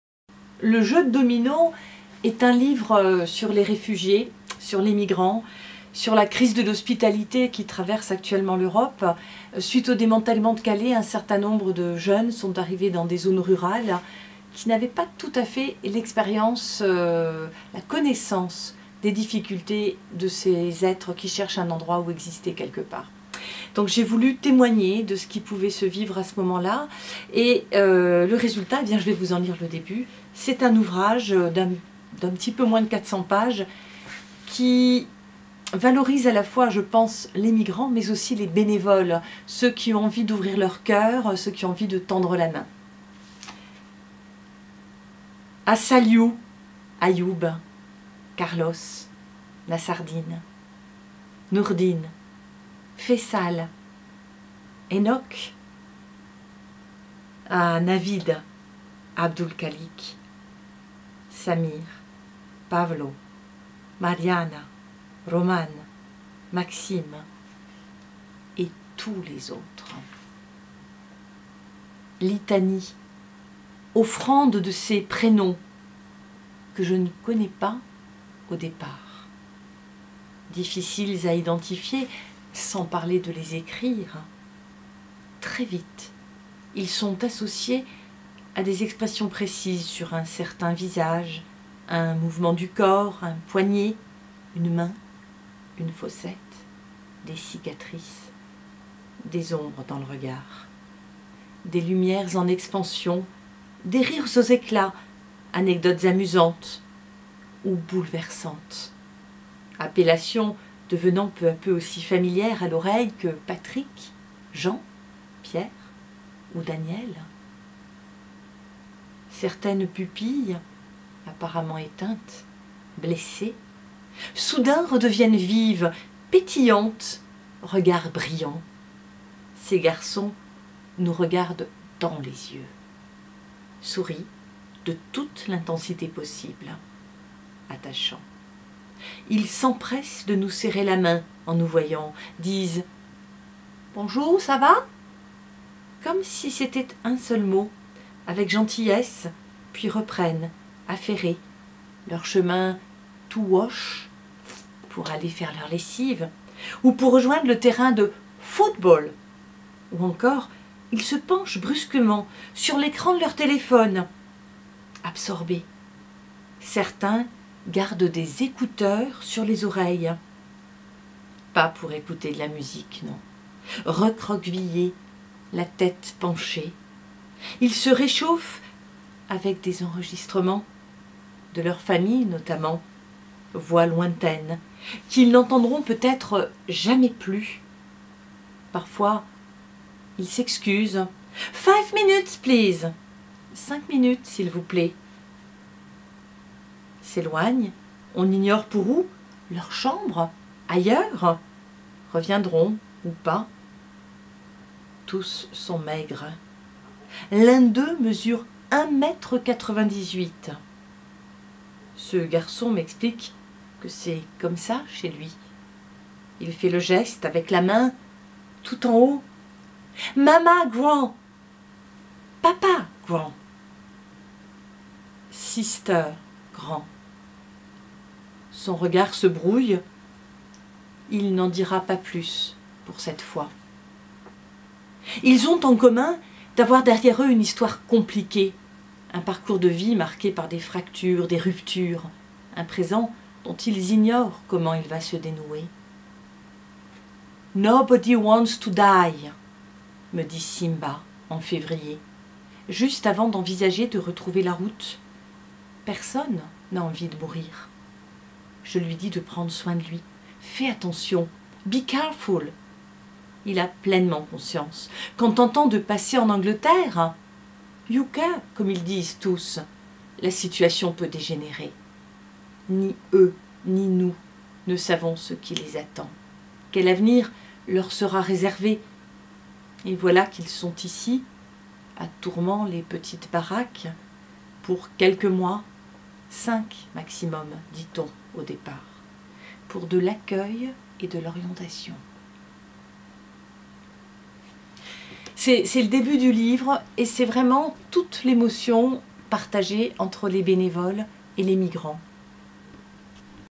"Le jeu de dominos" d'Anne Poiré
Lecture d'un extrait du début du livre "Le jeu de dominos" d'Anne Poiré, paru en novembre 2017 aux éditions Carmina. Un hommage aux réfugiés, aux bénévoles solidaires, un témoignage sur les CAOMI, suite au démantèlement de Calais en automne 2016. Lecture d'Anne Poiré. Couverture du livre : Anne Poiré et Patrick Guallino.